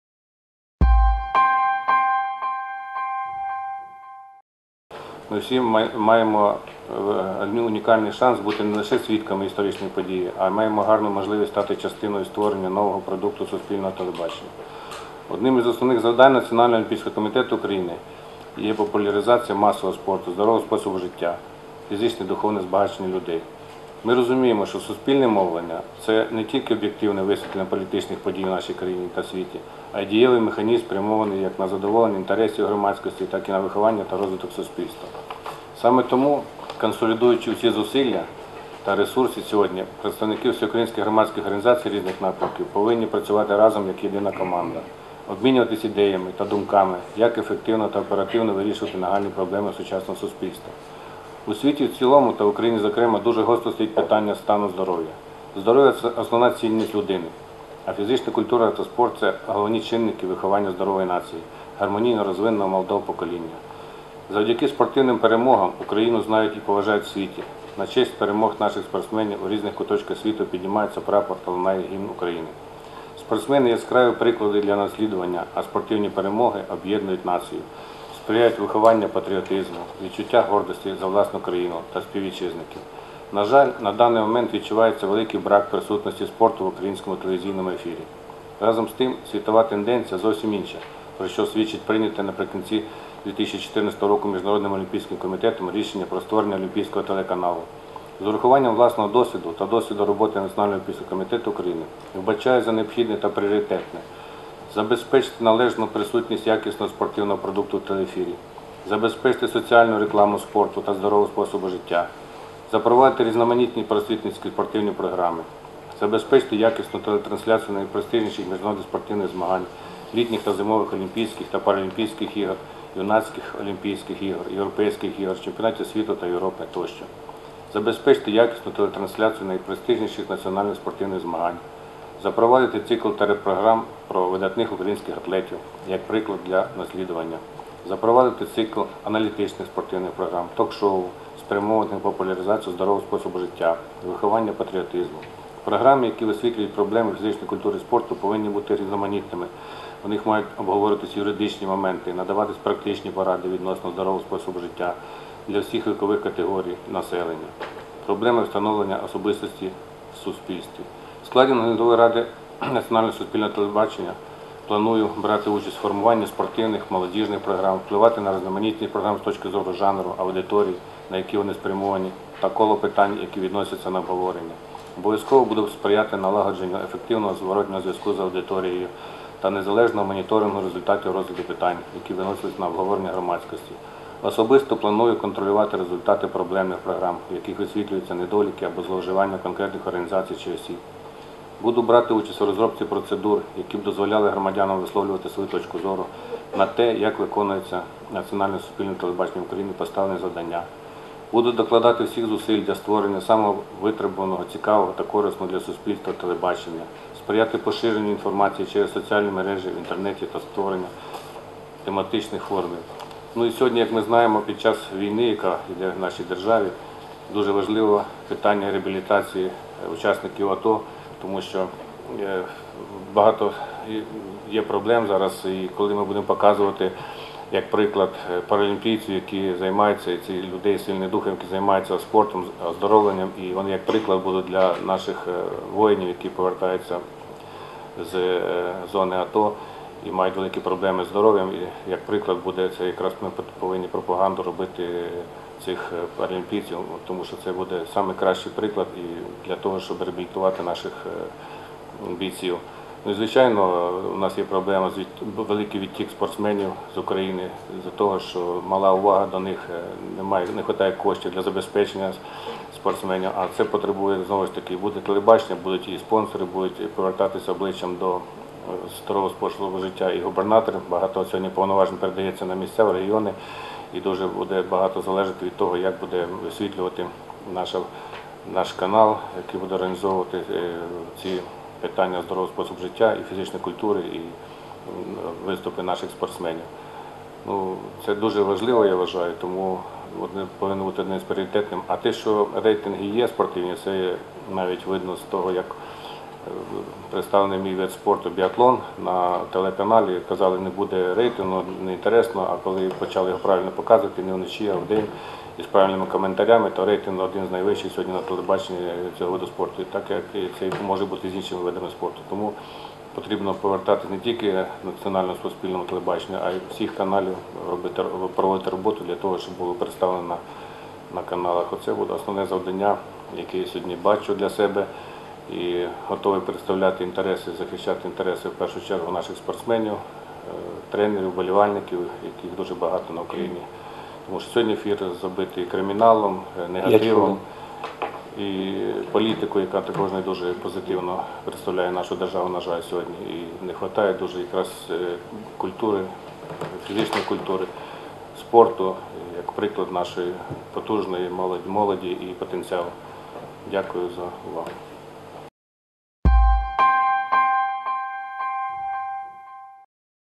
Виступ Володимира Бринзака
Виступ Володимира Бринзака (ВГО «Національний олімпійський комітет України») на конференції з обрання наглядової ради НСТУ в сфері спорту